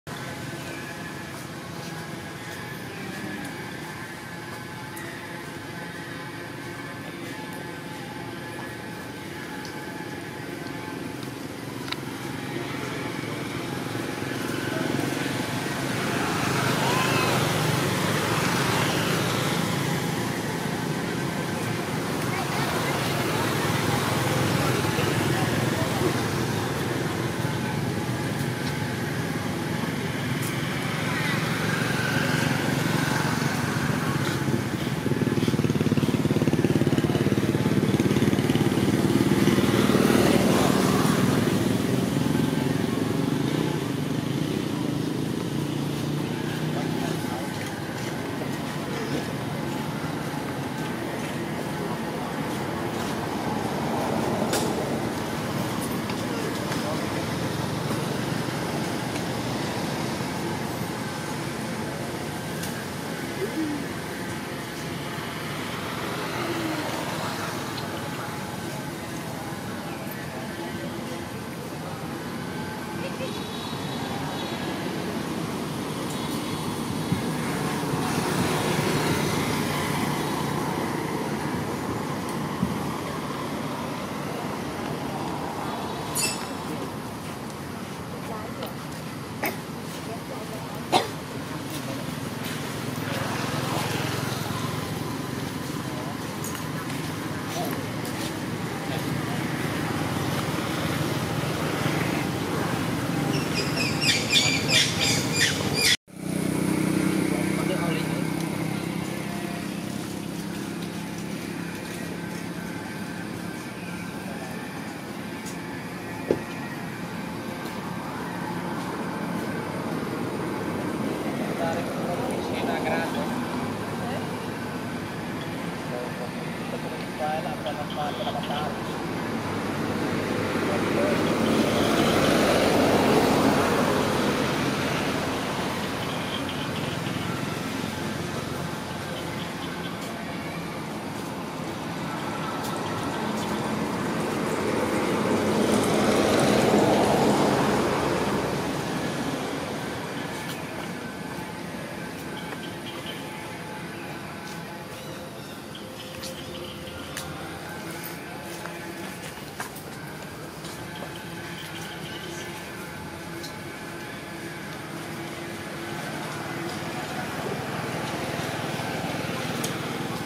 Jenet hungry and thirsty ! Looking to brother not share to jenet
Jenet hungry and thirsty now! Looking to brother not share to jenet
MONKEY POO is infant baby in the jungle, was found by villagers screaming alone, luckily he was brought to the village to taking care with milk and food supporting, Now Poo is a little bigger. This poor infant monkey poo will hand over to authority after growing up if he can run, climb and jump well.